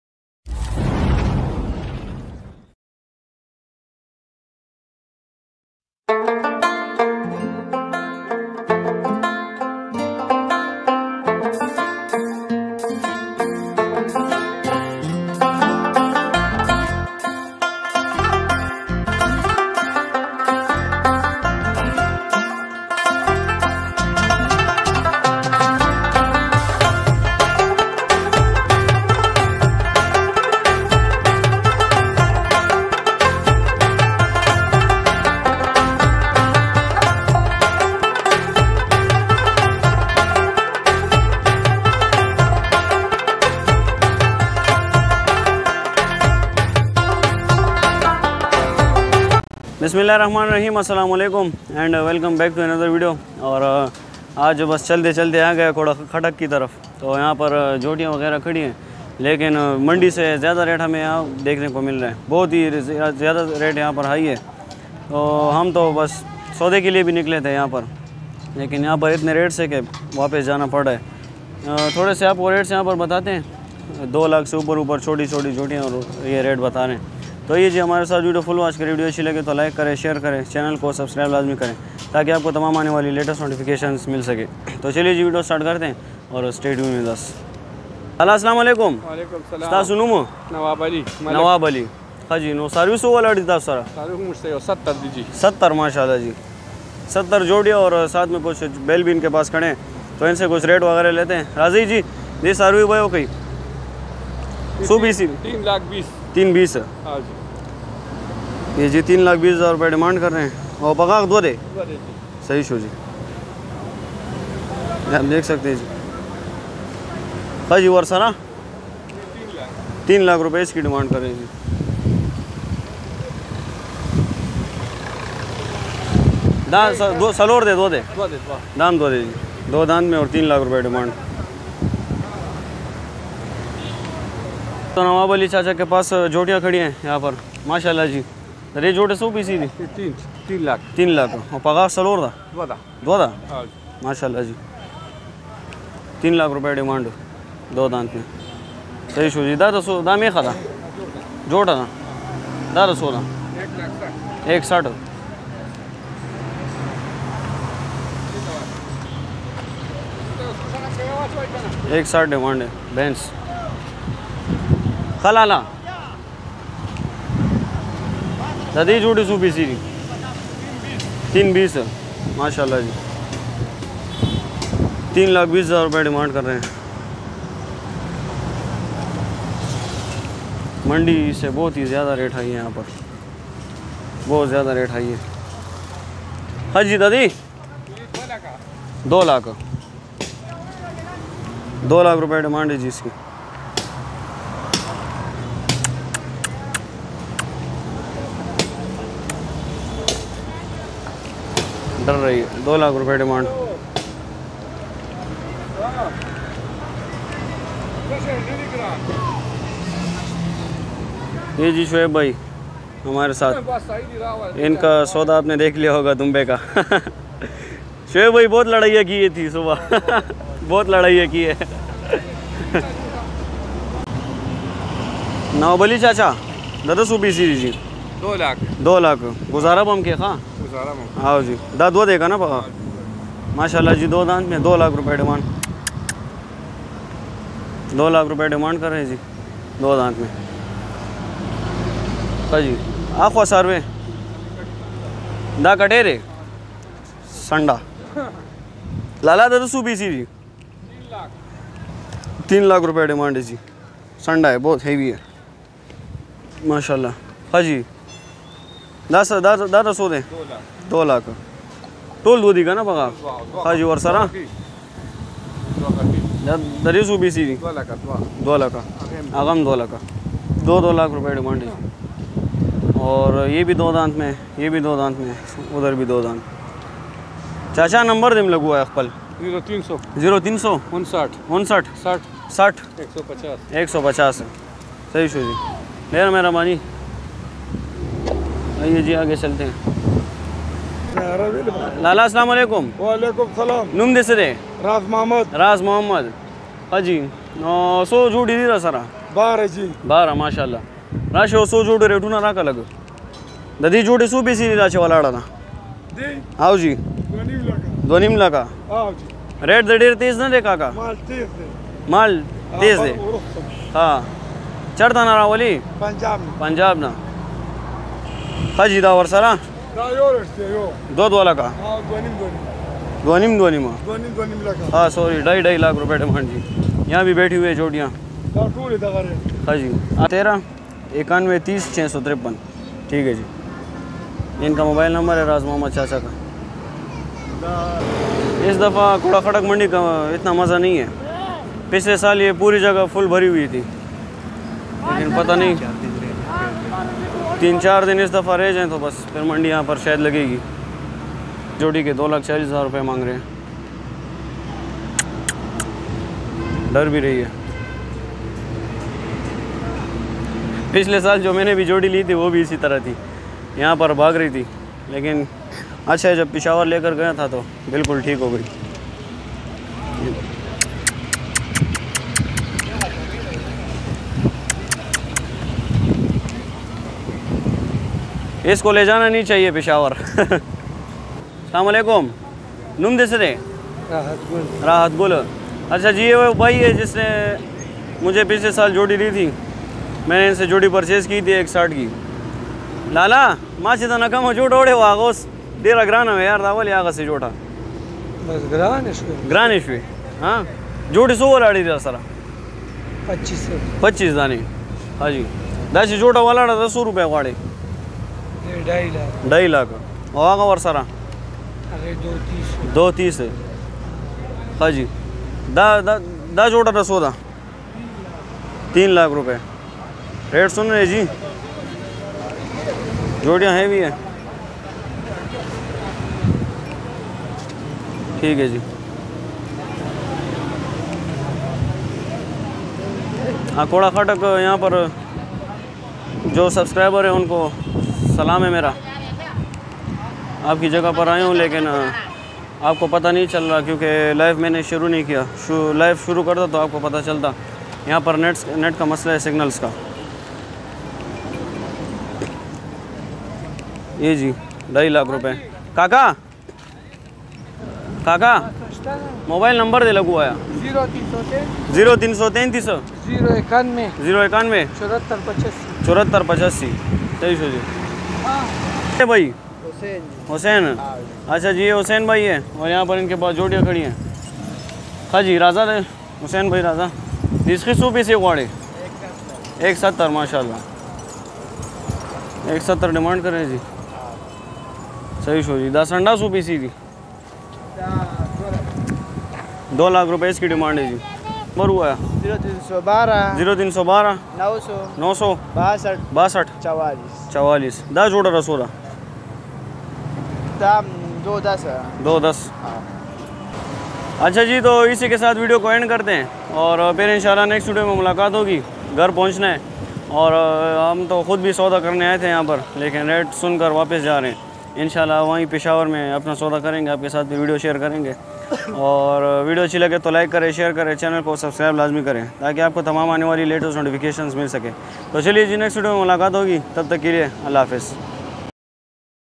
Akora Khattak Mandi Joti Rates || Maweshi Mandi Peshawar Pakistan || Cow Mandi 2022
hellow every one
today visit to Akora Khattak Mandi cow mandi 2022
KHAN'S TV OFFICIAL